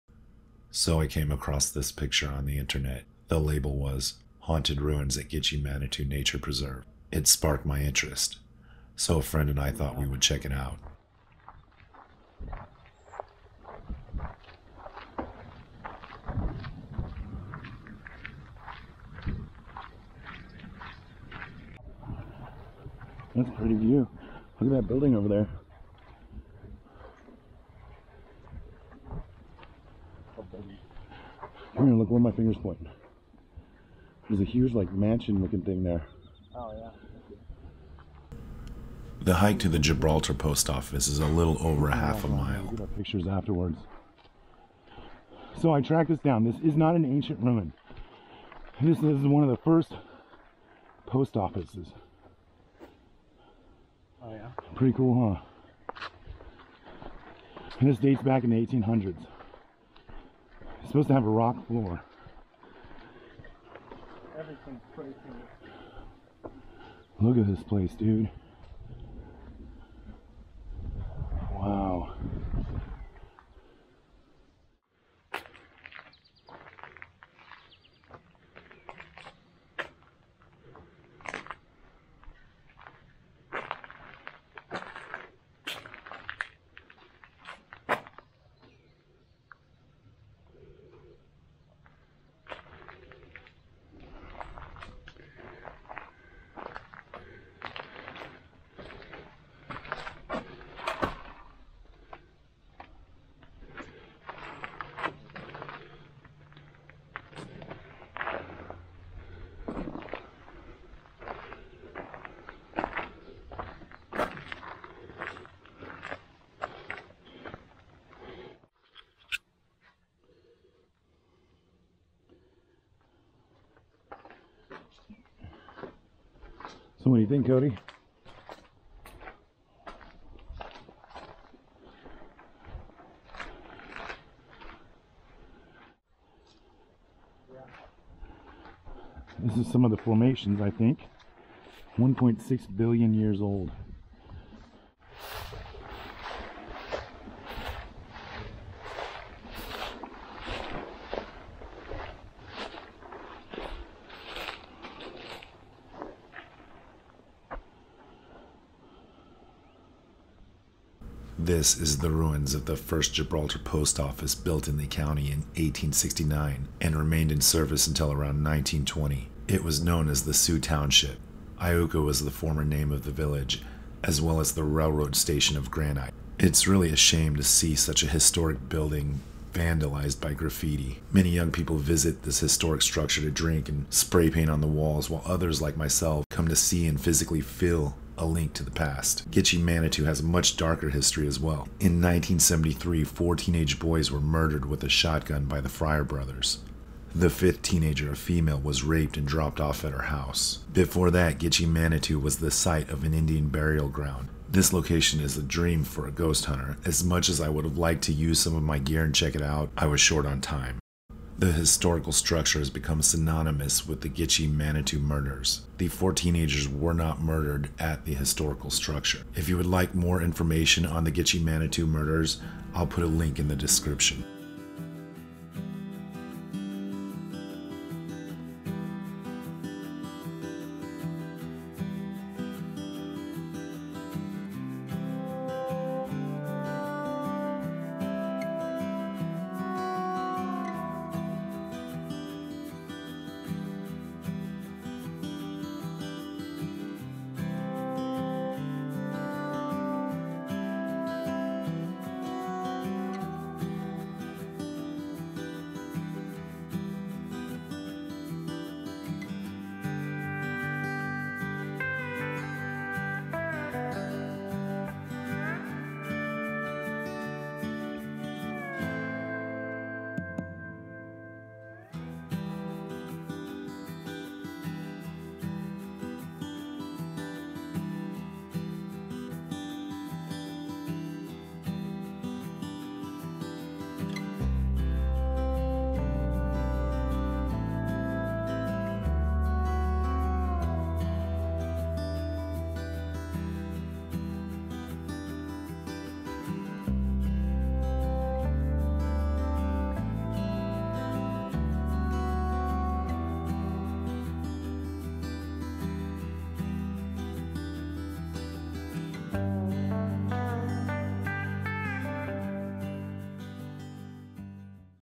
Hiking to the "Haunted Ruins" in Gitchie Manitou State Preserve
We drove 194 miles to go hiking at Gitchie Manitou State Preserve and see the "Haunted Ruins". The preserve is actually a very beautiful place to hike. If you would like more information about the Gitchie Manitou murders, Mobile instinct covers the details very well.